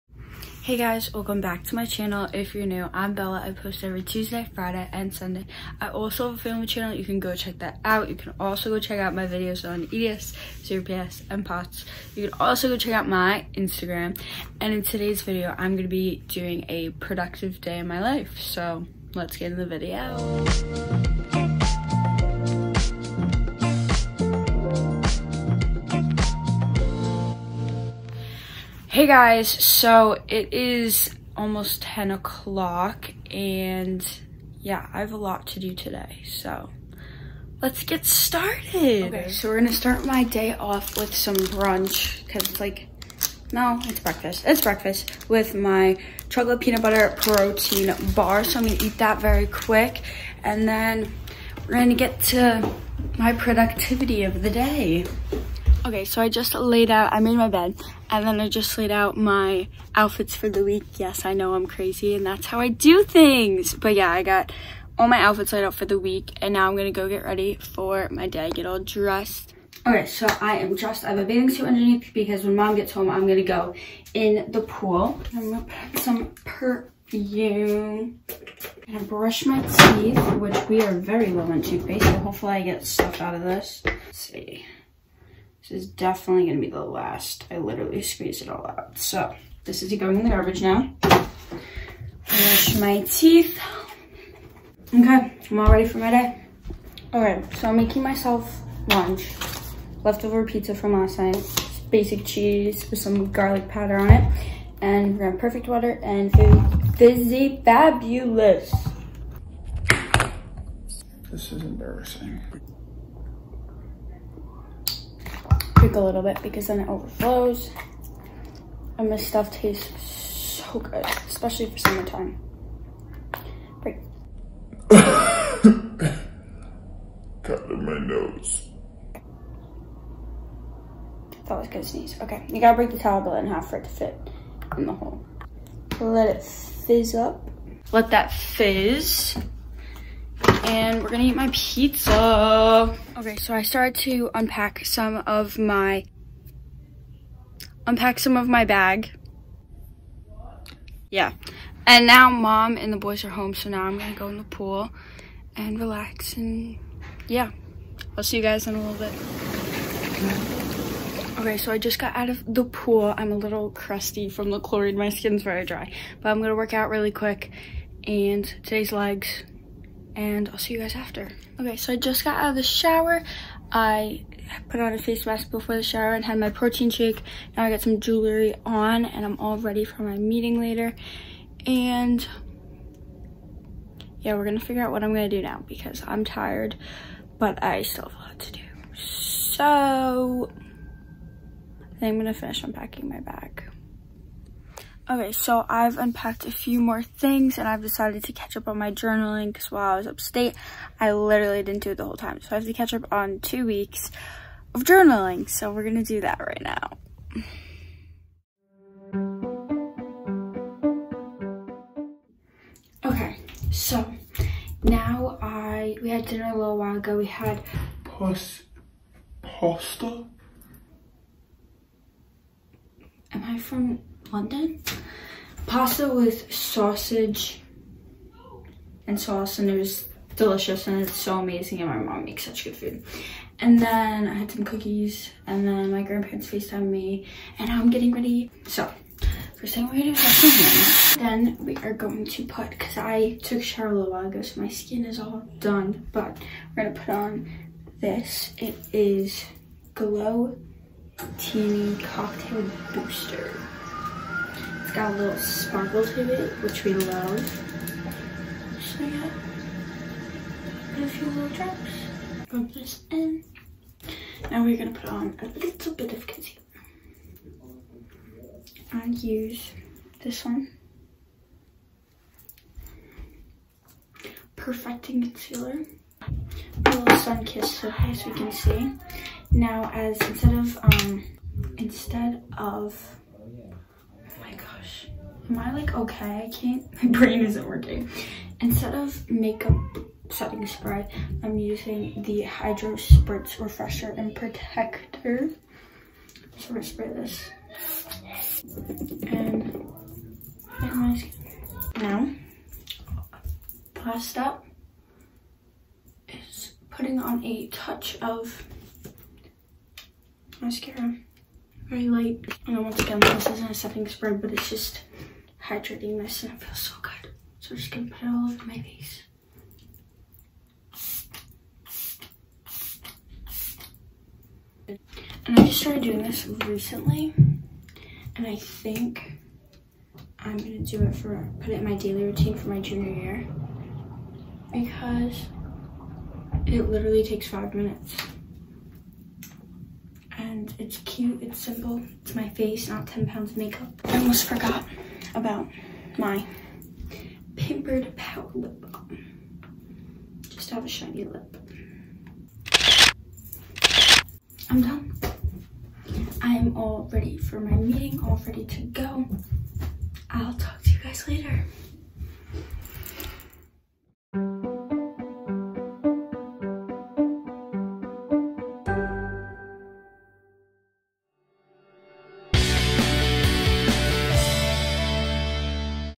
A Productive Day in my Life | 2022
Hey guys! If your new I’m Bella. I post every Tuesday, Friday, & Sunday.

Daily Workout:
Legs:
2 minute walk sit
100 front leg raises - bands
100 side leg raises - bands
100 back leg raises - bands
100 clams - bands
100 lungs - 15lb cowbell
100 squats -  15lb cowbell
100 donkey kicks - ankle weights
100 bridges - bands
100 90 degree bends - 30lb dumbbell
100 fire hydrants - ankle weights
100 side lunges - 15lb cowbell

Abs:
100 crunches
100 Russian twists - 15lbs cowbell
100 sit ups - 15lbs cowbell
100 toe touches  - ankle weights
100 flick taps - ankle weights
100 ankle touches
100 bicycle - ankle weights
100 hip dips

Arms:
2 minutes planks
100 push ups
100 pull downs - 15lb dumbbells
100 side raises - 15lbs dumbbells
100 front raises - 15lbs dumbbells
100 curls - 30lbs dumbbell
100 chicken wings - bands (forward straight)
100 row - 15lbs dumbbells
100 curls - band
2 minute arm hold

Cardio:
100 high knees
100 butt kicks
100 jumping jacks
100 burpees
100 jump squats
100 jump lunges